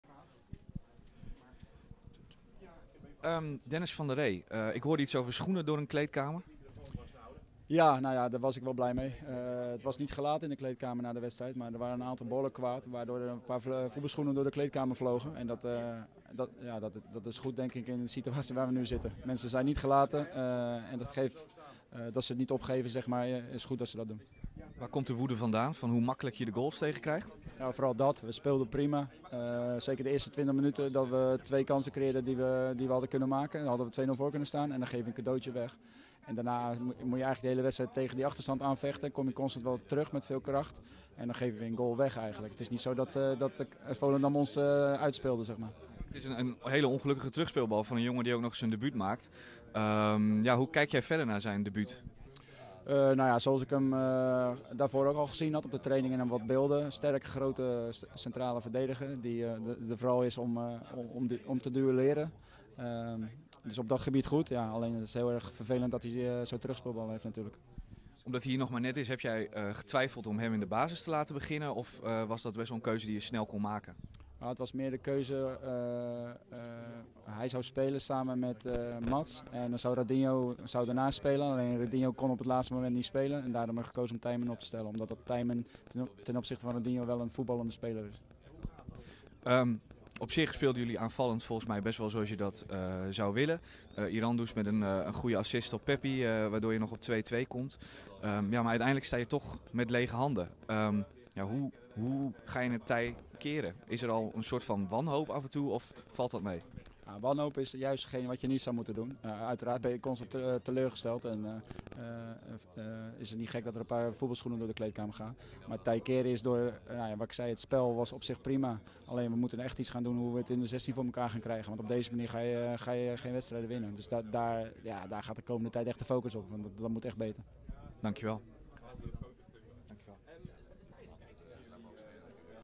Van der Ree: ‘Schoenen vlogen door de kleedkamer’
Trainer Dennis van der Ree na afloop van FC Volendam - FC Groningen (3-2)